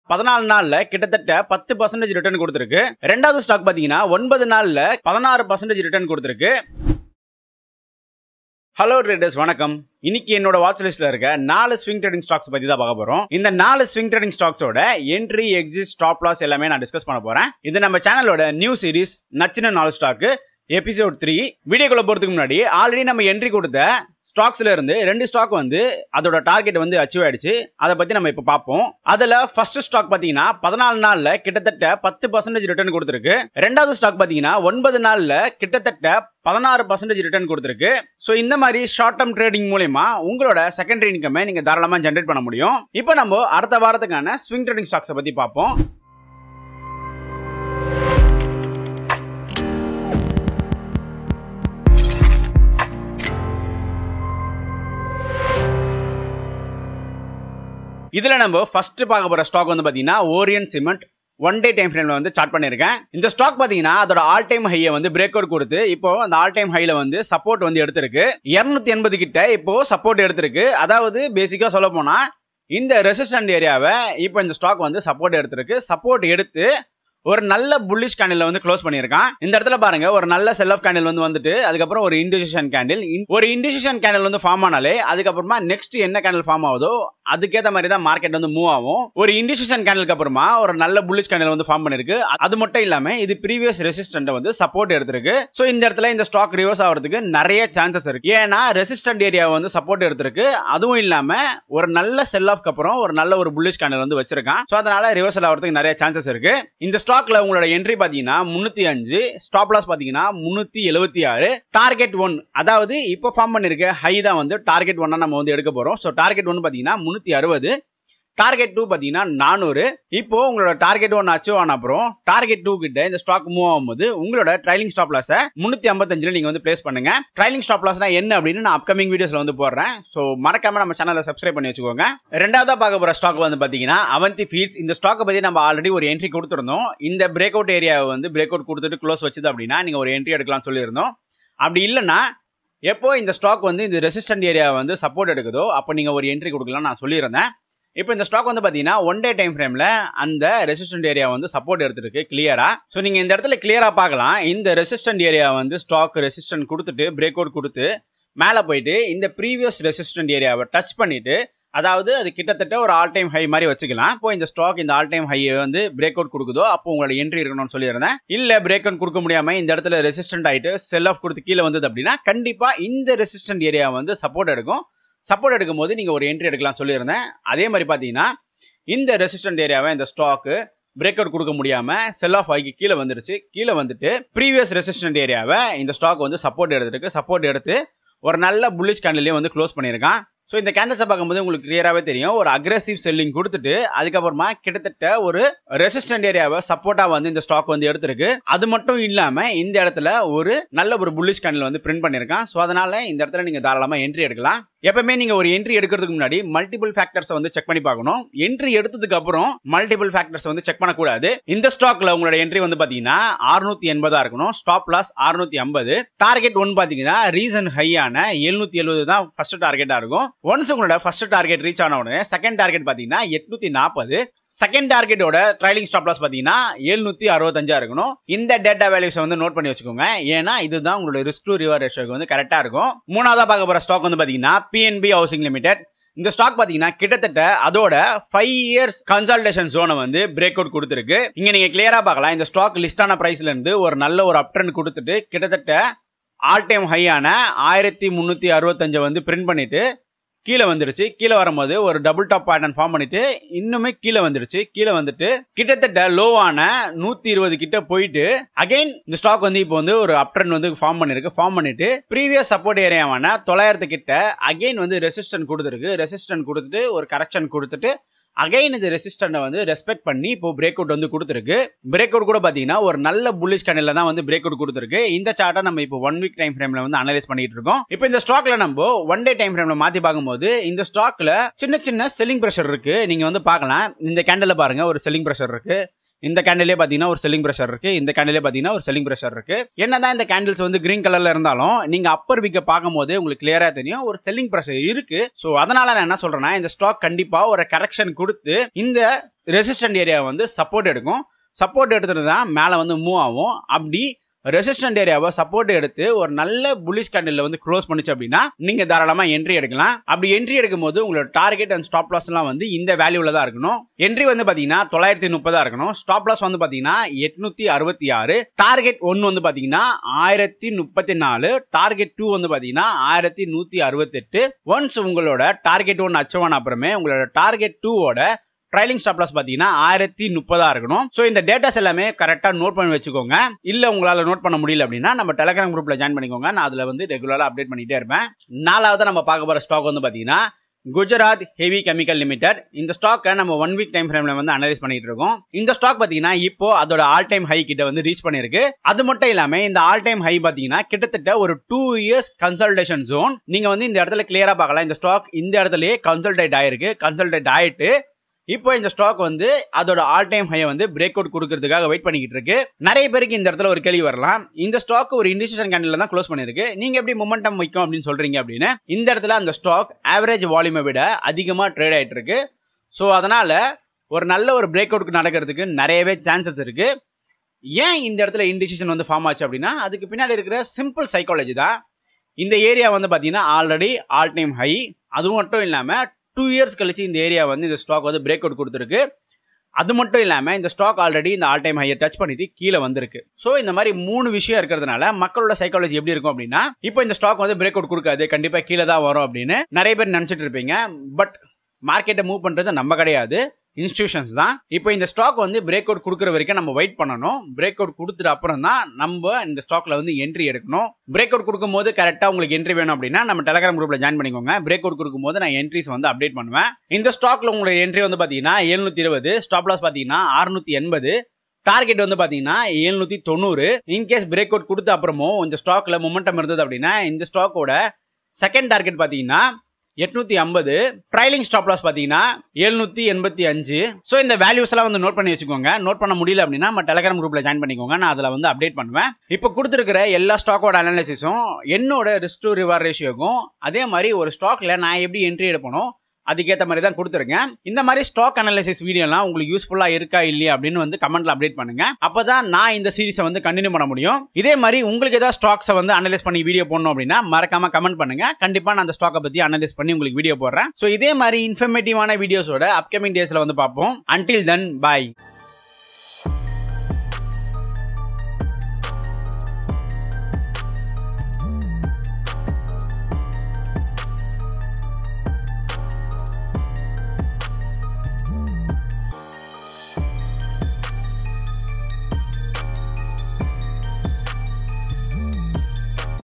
Swing Trading Secrets: 4 Breakout Stocks to Buy Now (N4S Episode 003)
"Discover hidden gems in the market! Join us for N4S Episode 003 as we reveal 4 breakout stocks poised for significant gains. Learn our proven swing trading strategies and unlock the secrets to consistent profits. Don't miss this opportunity to add these potential winners to your watchlist."